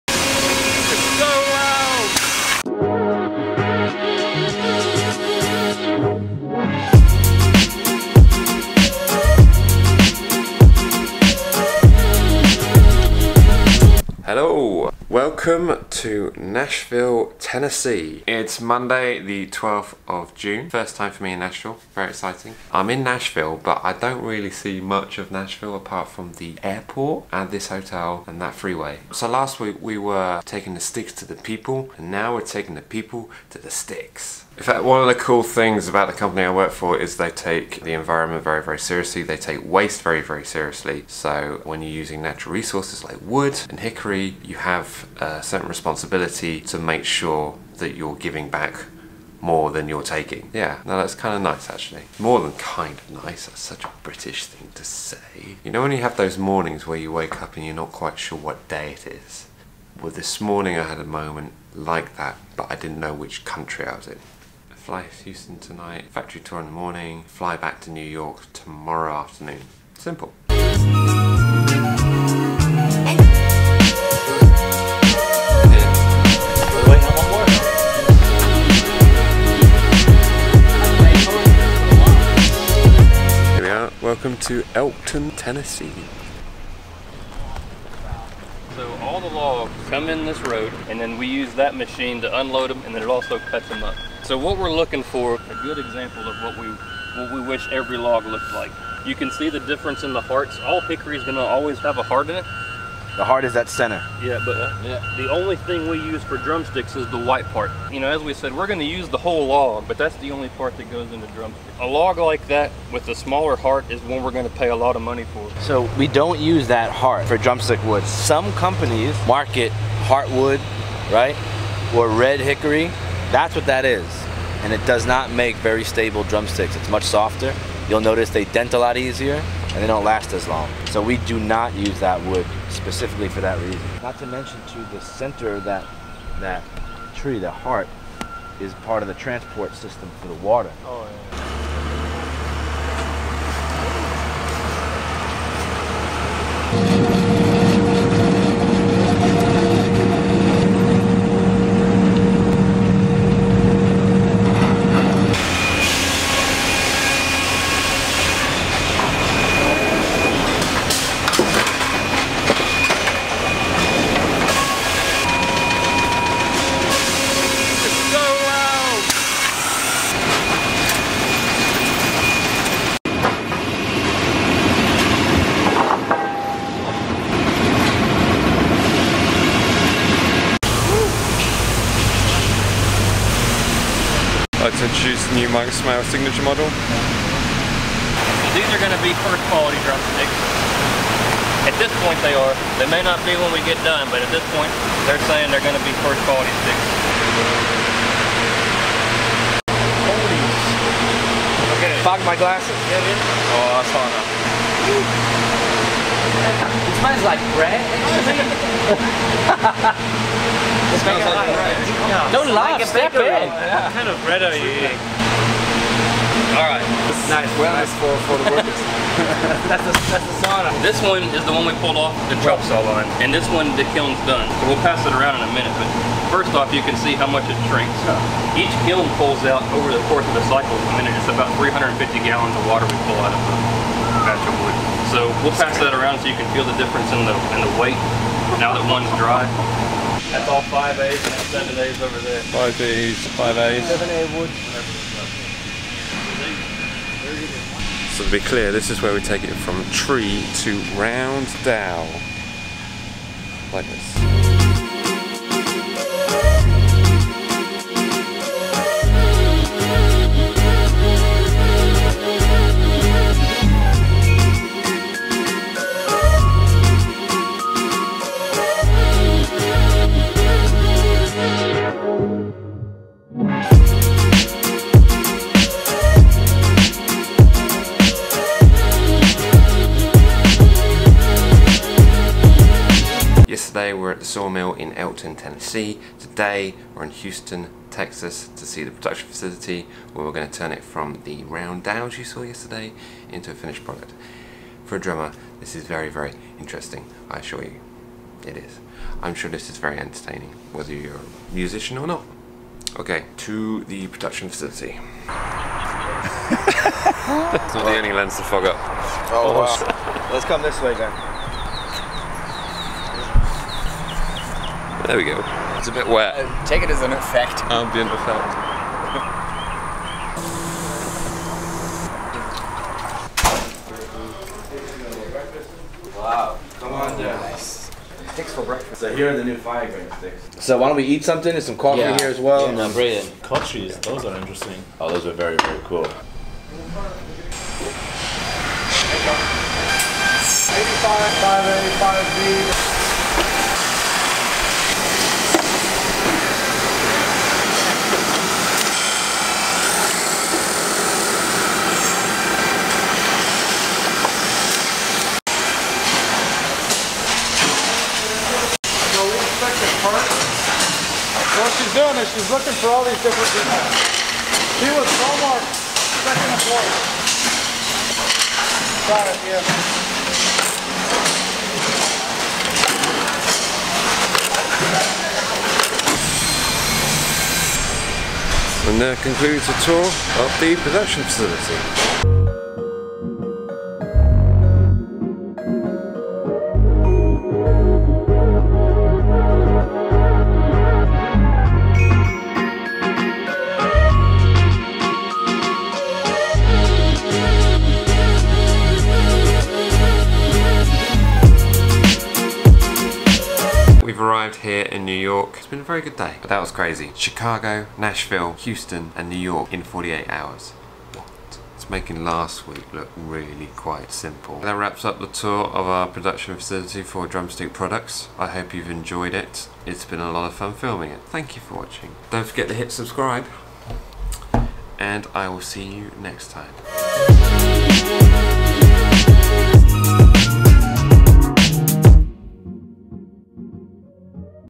TREE to STICK - How DRUMSTICKS are made
We take the full tour to see the sawmill in Elkton Tennessee and the production facility in Houston Texas..TREE to STICK - Visiting Promark
for more details on Promark and the commitment to the reforestation of FIVE trees for every one used, go